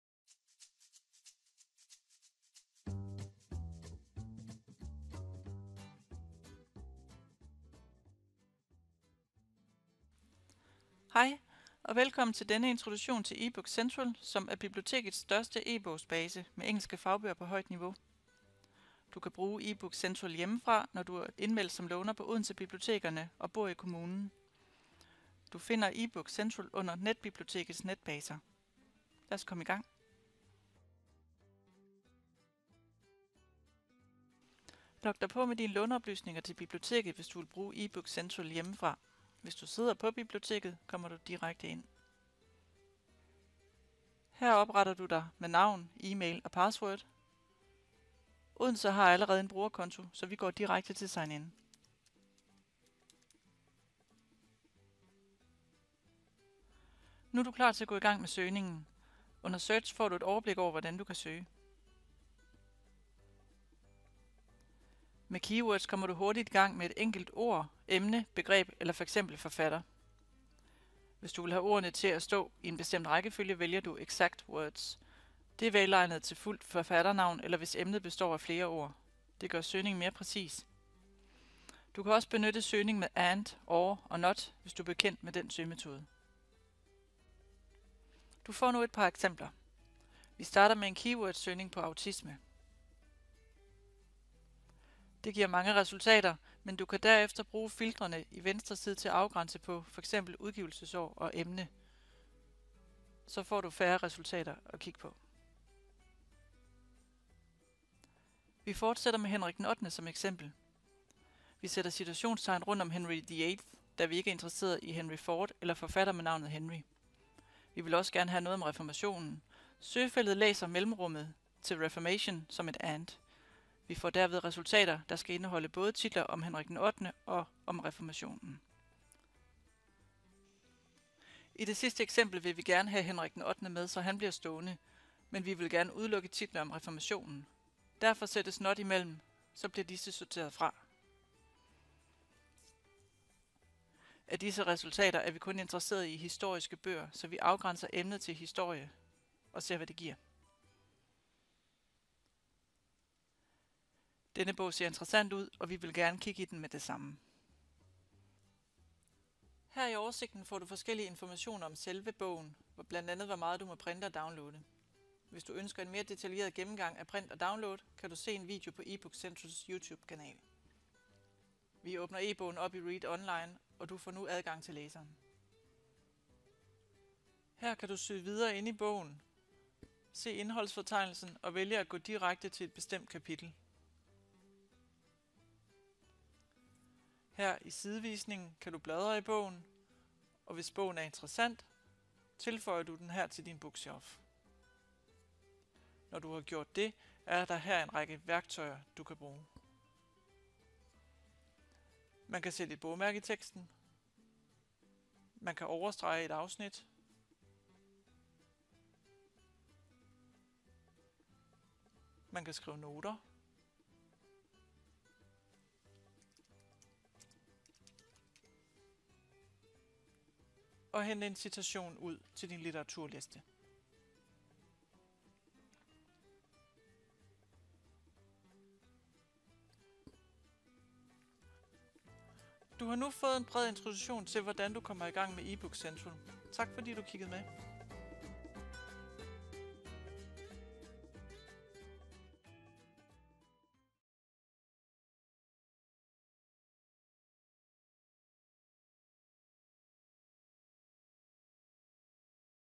Ebook Central - Sådan gør du!
I denne guide bliver du introduceret til brug af Ebook Central - et kæmpe online bibliotek for fagbøger.

Ebook Central indeholder:

-    Fagbøger fra cirka 180 forlag herunder Aarhus University Press

-    Fagbøger inden for økonomi, humaniora, sprog, religion, kunst herunder musiske fag, litteratur, naturvidenskab, IT  & teknik, medicin, historie og samfundsvidenskab